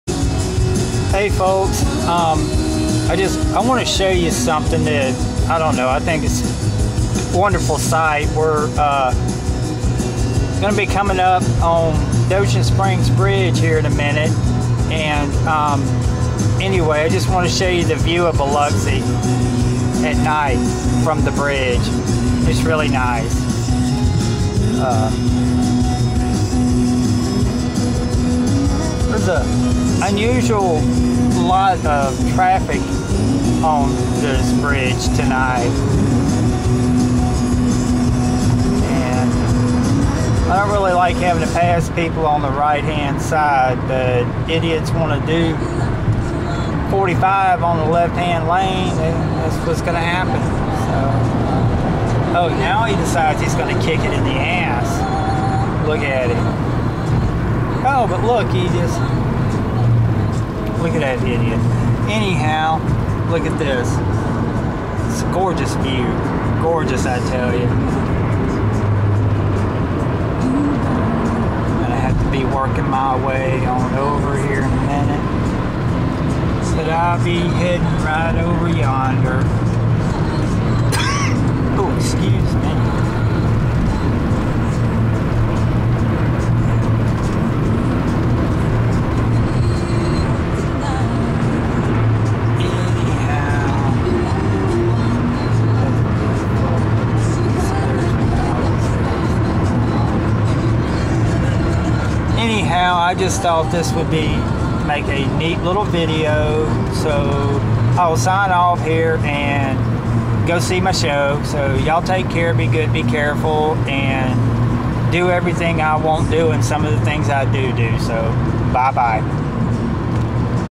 Biloxi, at night
This clip is a ride across the Biloxi-Ocean Springs at night and shows what Biloxi looks like at night, WHILE crossing the bridge. Just watch it, you'll see.